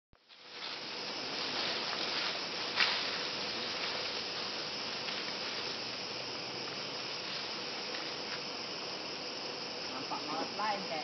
Orangutan with baby swinging in tree - short
Short video of Orangutan swinging in the trees, see my other video for more.

Taken in Somatra's Bukit Lawang, while we were on a overnight Jungle trek.